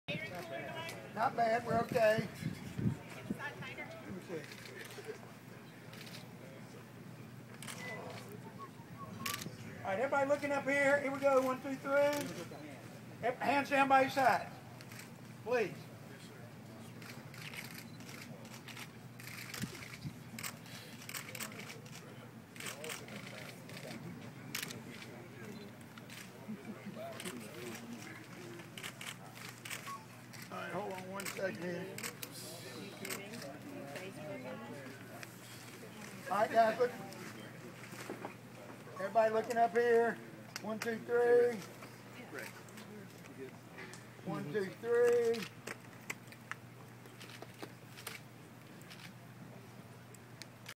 Player Photo Shoot for ACC Football Kickoff
Roddy Jones and Logan Walls pose for a photo with players from all 12 ACC schools in Pinehurst, NC.